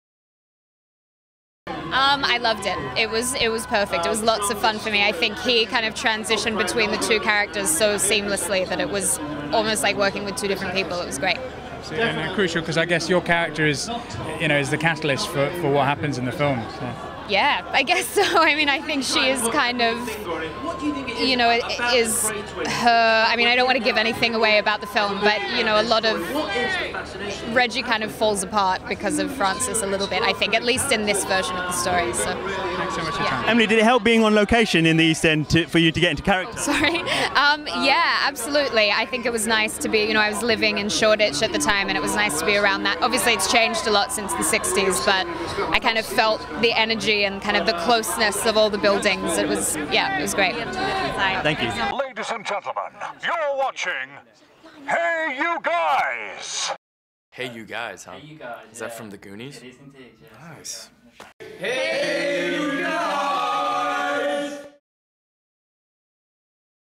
Emily Browning Interview - Legend World Premiere
Cassam Looch from HeyUGuys interviews Emily Browning for her movie Legend and the World Premiere for the film in London.

Legend stars Tom Hardy as both famous East end gangsters Ronald Kray / Reginald Kray.